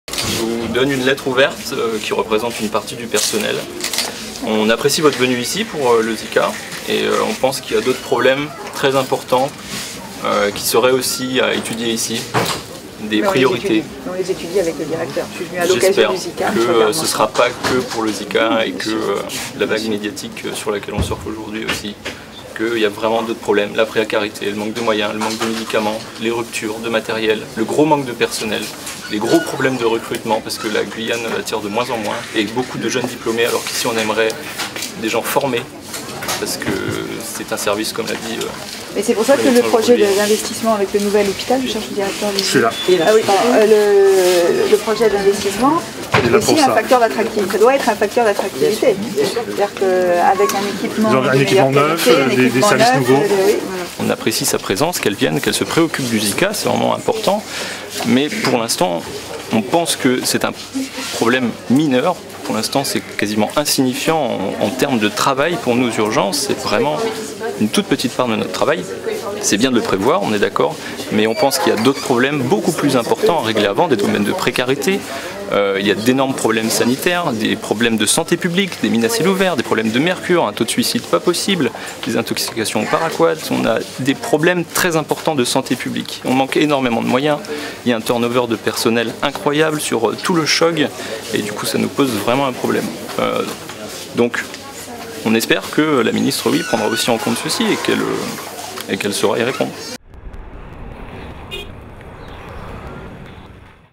Un infirmier évoque à Marisol Touraine des problèmes plus important que le ZIKA
Lors du passage de la Ministre de la Santé et des Affaires Sociales au Centre Hospitalier de l'Ouest Guyanais, un infirmier représentant des médecins urgentistes, remet une lettre ouverte à Marisol Touraine sur d'autres problèmes liés à leur service...